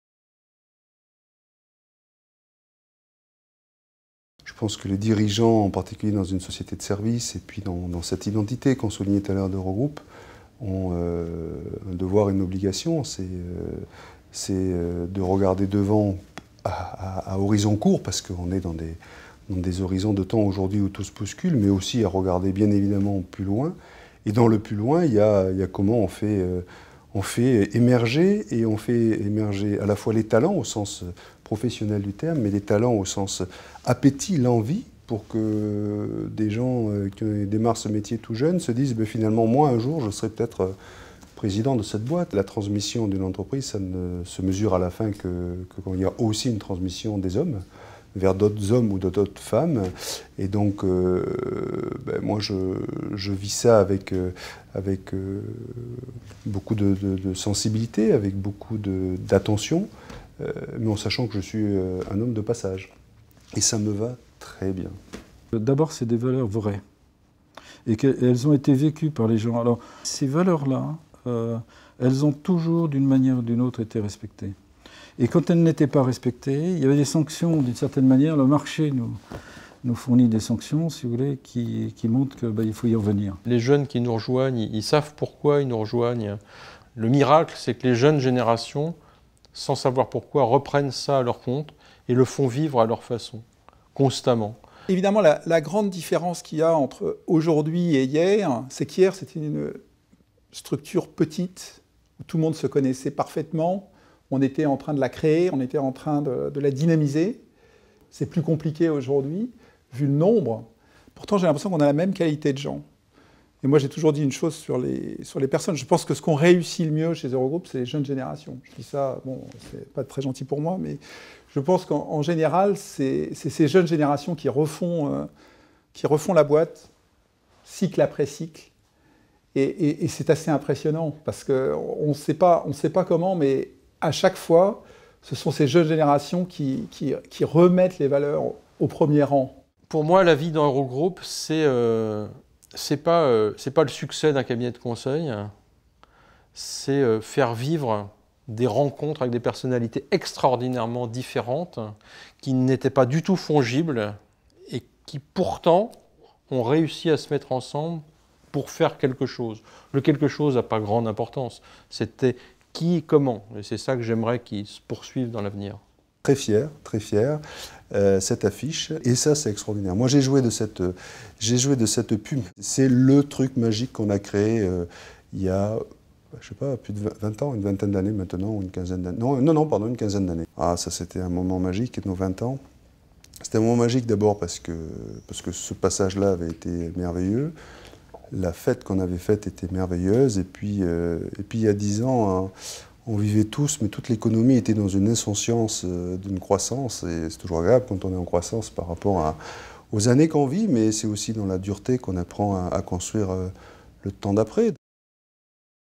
Eurogroup Consulting a 30 ans : ses dirigeants en parlent
Jean-François Deney, Francis Rousseau, Gilles Bonnenfant et Alain Guyot, les anciens et actuels dirigeants d’Eurogroup Consulting, témoignent des valeurs qui ont préfiguré à la création et au développement du cabinet.